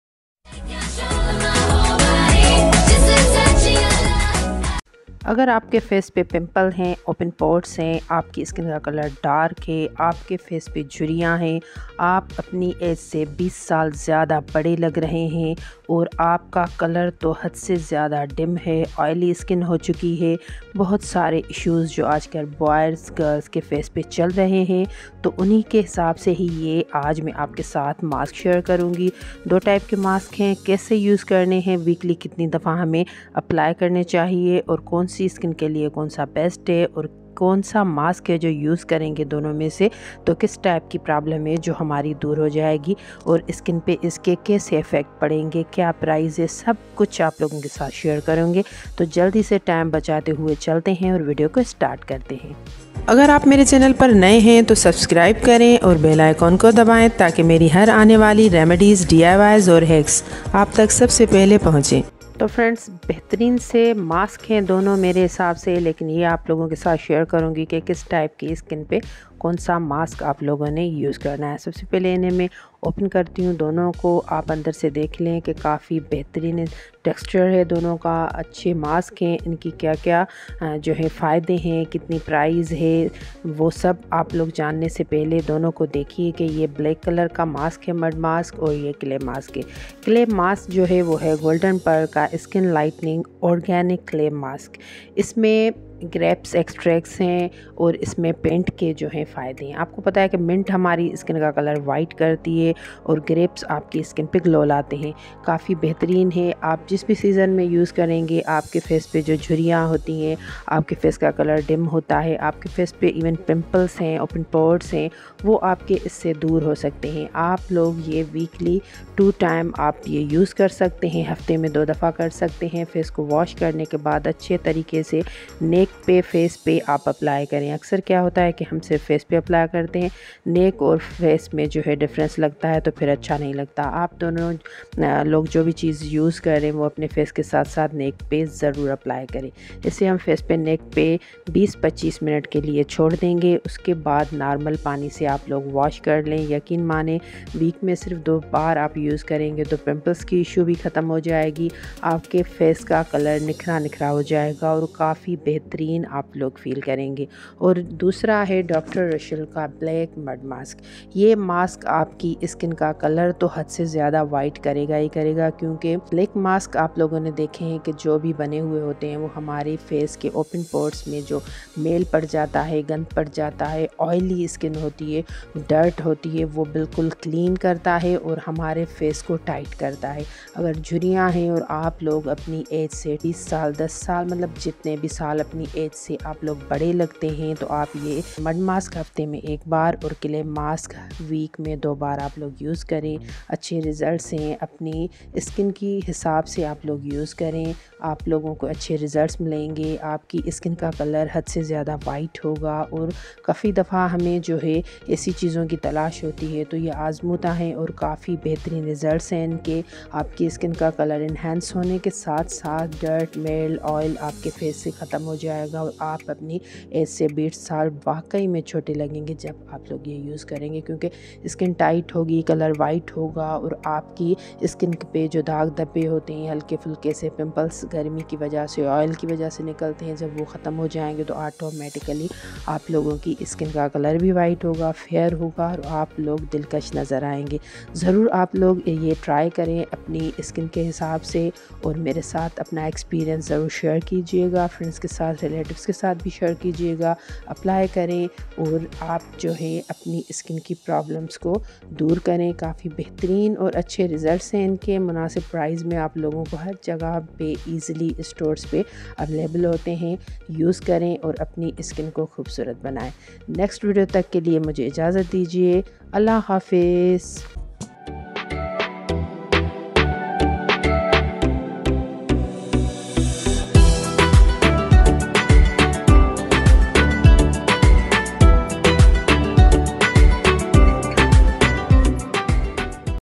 Skin Lightening Organic Clay And Mud Mask For All Skin Types | Beauty Tips
Give your skin a breath of freshness with Golden Pearl Skin Lightening Organic Clay Mask. With the goodness of grape extracts and peppermint, it exfoliates and revitalizes the appearance of your skin. Mineral oil, yogurt extract and aloe vera provide your skin an ultra- boost of hydration, while Kaolin Clay tones the skin complexion, so your skin looks brighter and clearer.

Black Mud Mask Whitening

DR RASHEL collagen & charcoals black mud mask works to reduce the oily secretions and is recommended for those who suffer from the wide pores